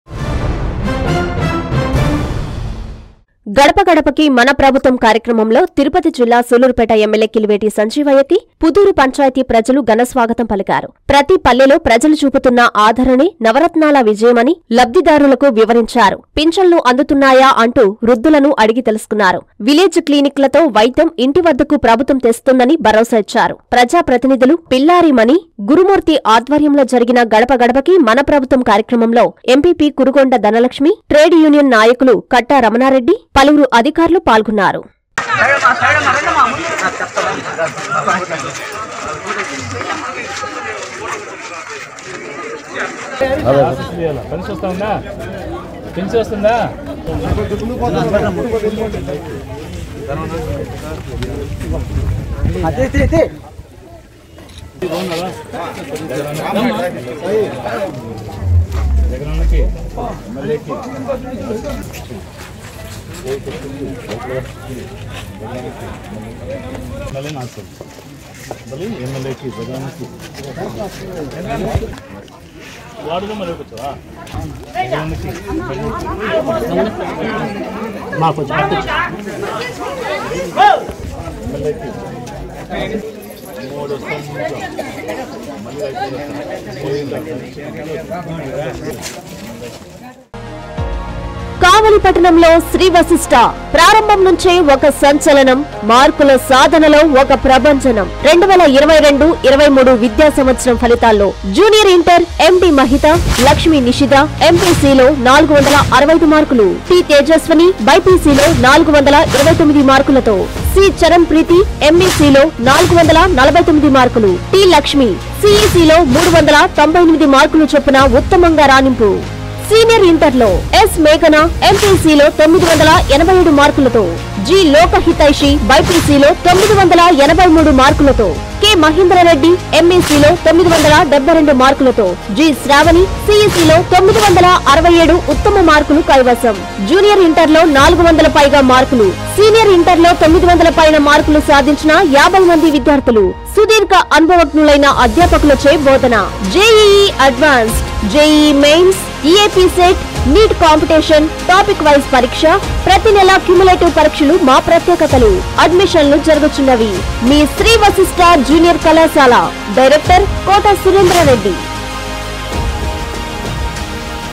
ప్ర‌జాధ‌ర‌ణే... న‌వ‌ర‌త్నాల విజ‌యం
పుదూరులో గ‌డ‌ప గ‌డ‌ప‌కు మ‌న‌ప్ర‌భుత్వం
ఎమ్మెల్యే కిలివేటికి ఘ‌న‌స్వాగ‌తం ప‌లికిన ప్ర‌జ‌లు